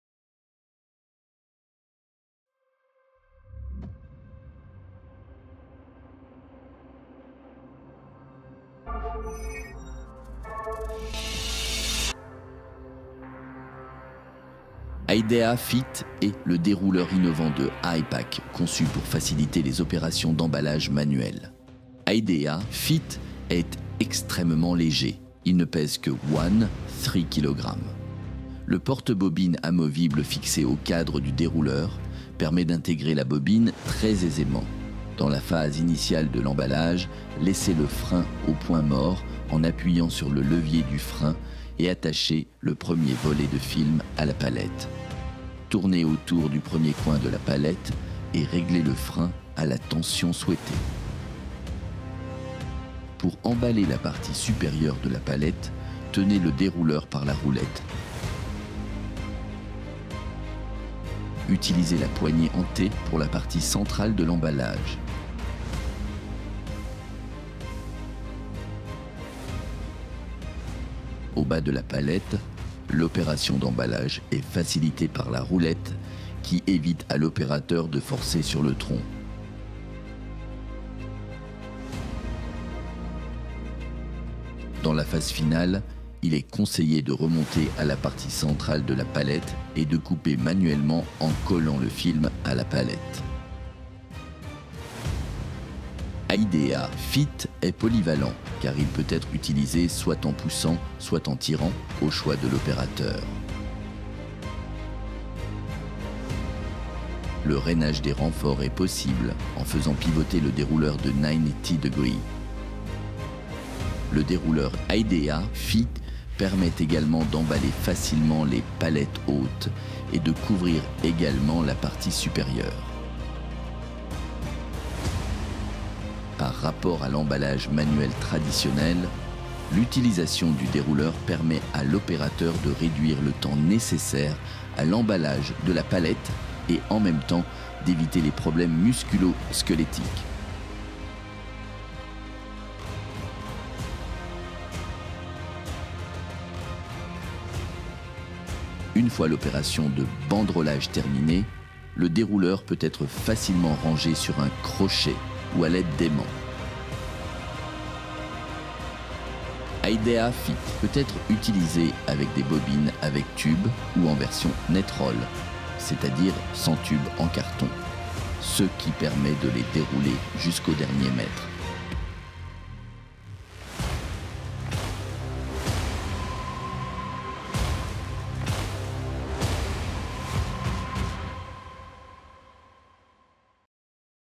Tutorial HI.DEA FIT FR
HI.DEA FIT est le nouveau dérouleur de HIPAC conçu pour révolutionner les techniques d'emballage manuel en apportant des améliorations majeures à la santé et à la sécurité des opérateurs.

Voici les principales caractéristiques du dérouleur:

- Ergonomique
- Léger
- Polyvalent

Dans la vidéo, vous trouverez toutes les instructions pour utiliser le nouveau dérouleur HIPAC.

Participez à la révolution de l'emballage manuel !

Traduit avec DeepL.com (version gratuite)